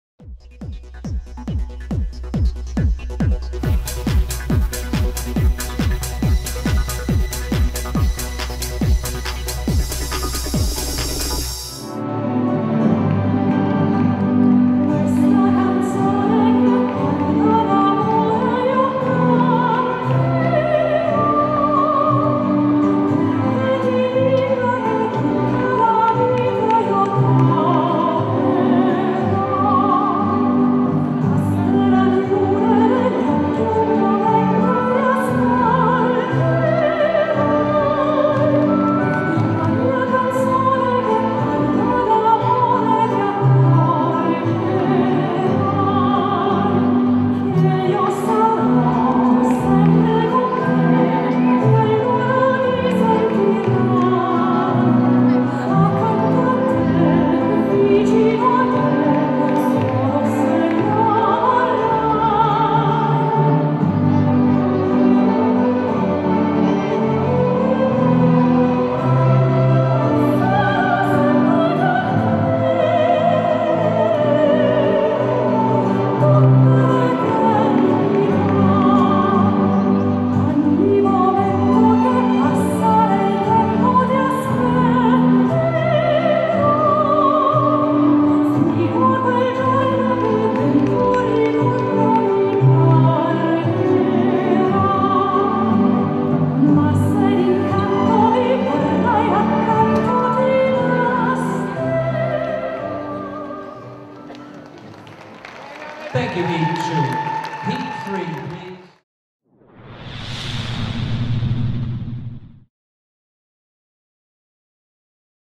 Usmanov - Rymareva, BLR | 2013 European Standard R2 VW
Roman Usmanov - Valeryia Rymareva, BLR,dance the second round Viennese Waltz at the 2013 WDSF European DanceSport Championship Standard in Aarhus, DEN, on 20 April.